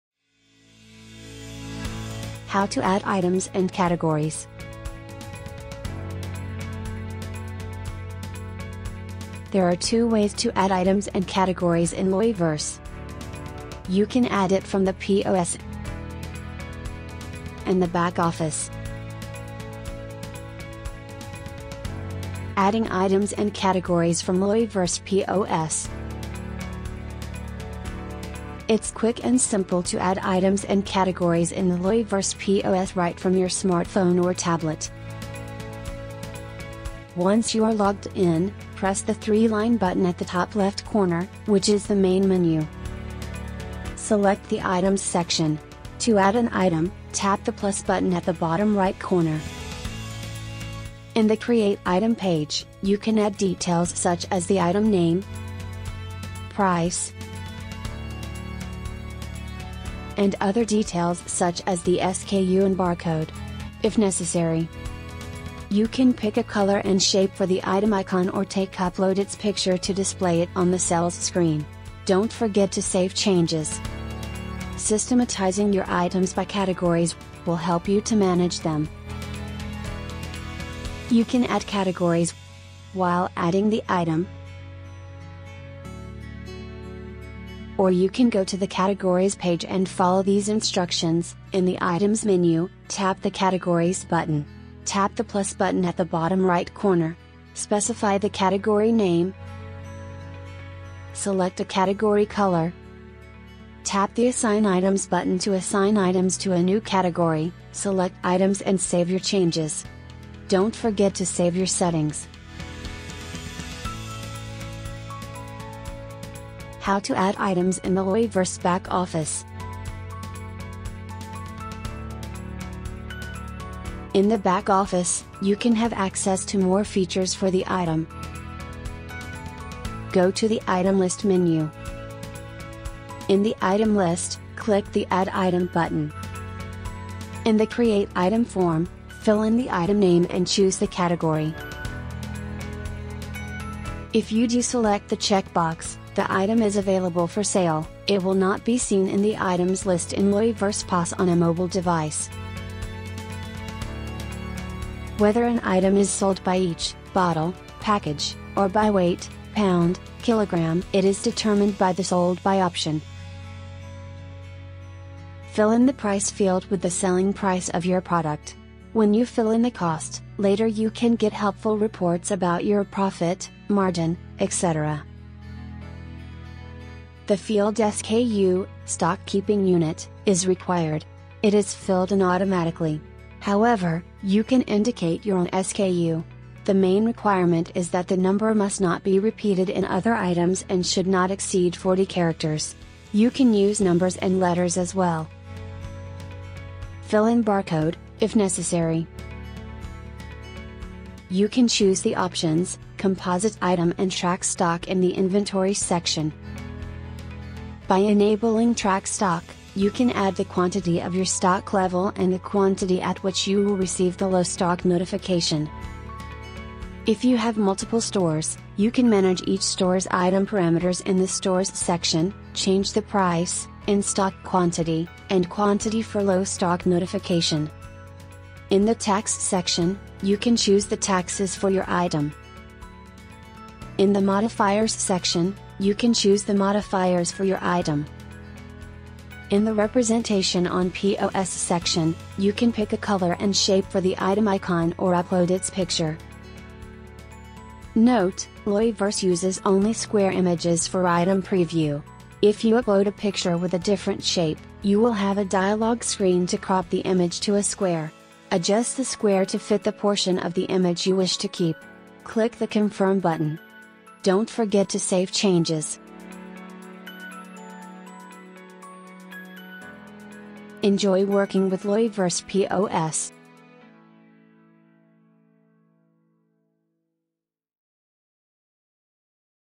How to Add Items and Categories in Loyverse POS and Back Office
0:00 Intro
0:22 Adding items and categories in Loyverse POS
1:56 Adding items and categories in Loyverse Back Office

There are two ways to add items and categories in Loyverse. You can add it from the POS and if you do this in the Back Office, you can have access to more features for the item.